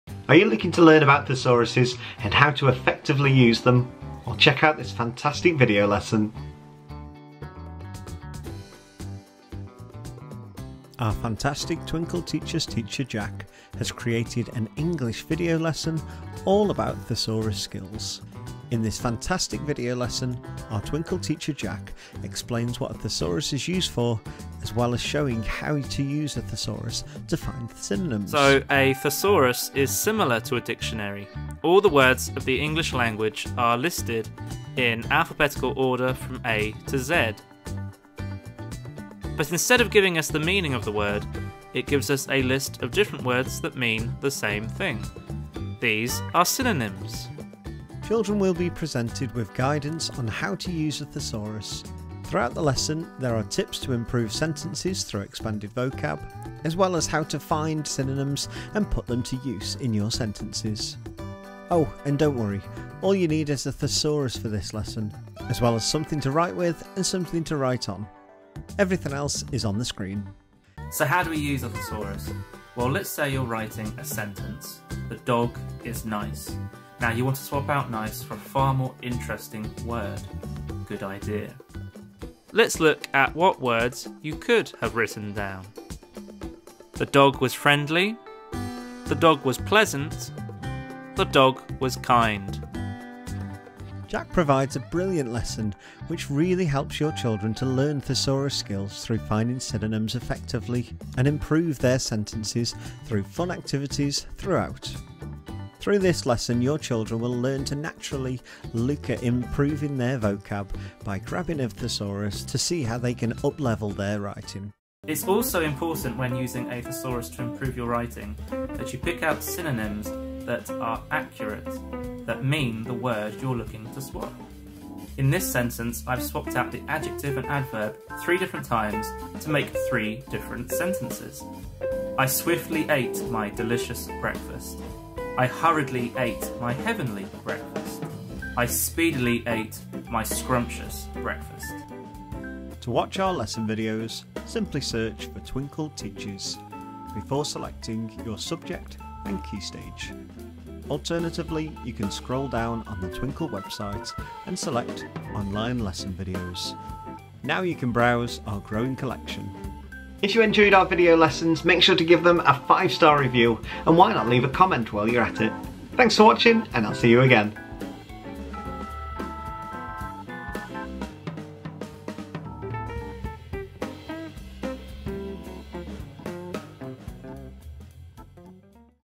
How to Use a Thesaurus | KS2 Video Lesson Preview
In this video, Twinkl Teacher James shares Jack's fantastic lesson on how to use a thesaurus. Children will learn valuable thesaurus skills to help with their writing

0:00 Introduction
0:11 Lesson Series Overview
1:11 Lesson Preview
2:22 Where to Find Our Lessons